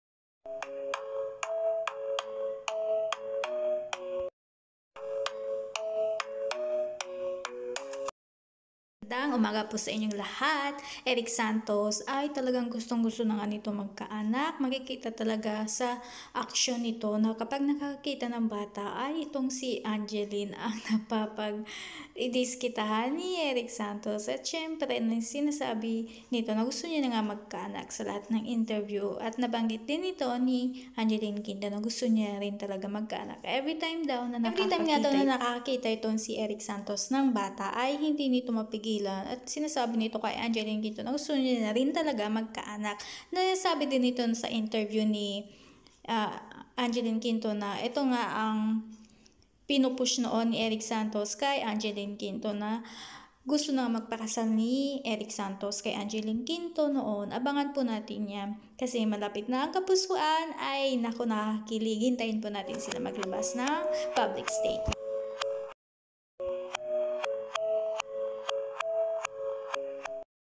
NANABIK NA SI ERIK NA MAGKA ANAK GANUN DIN SI ANGELINE "THE FEELING IS MUTUAL "
SANA DAW AY MAGKA ANAK NA SILA NI ERIK SANTOS AT ANGELINE QUINTO